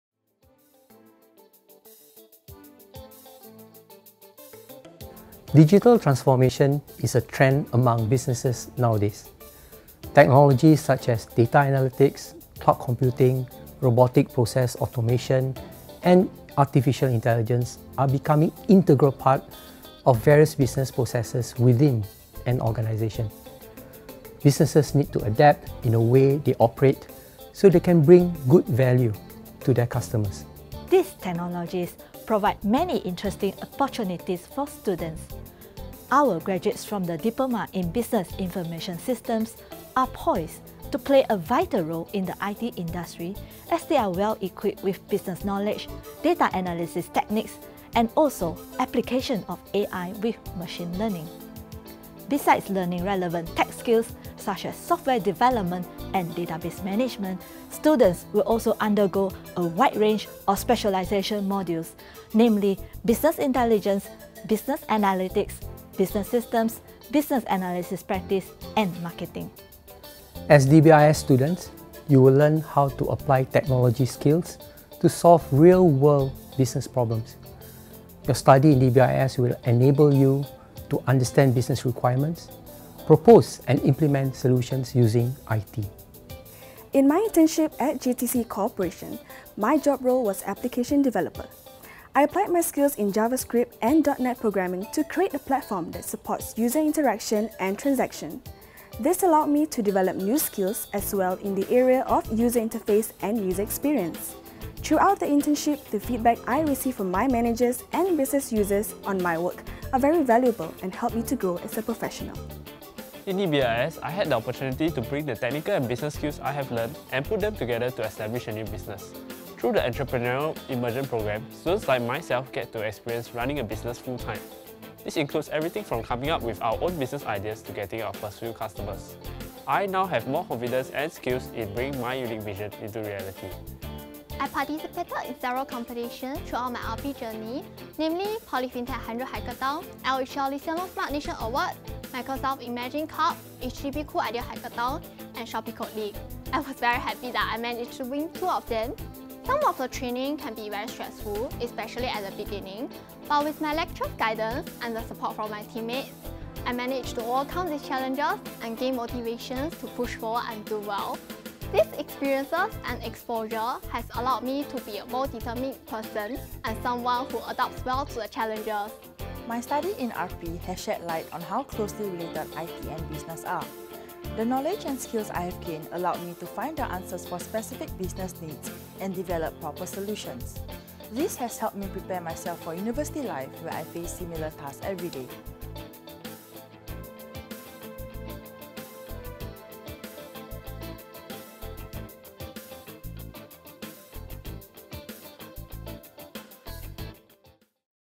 Diploma in Business Information Systems (DBIS)
Learn how to apply technology skills to solve real world business problems. Master a combination of technology skills, core business knowledge and business and data analysis techniques. Gain insights that will enable you to understand business requirements, propose and implement solutions using IT.

In addition to theoretical learning, you will be exposed to world class software technologies such as Microsoft .NET and SAS. We partner leading companies like Microsoft, GIC, Accenture, StarHub, Salesforce Singapore, and SAS Institute to provide you with valuable experiences through internships and industry projects.

By the time you graduate, you will have the confidence and expertise needed to build a successful IT career.

More information about the Diploma in Business Information Systems (DBIS) can be found in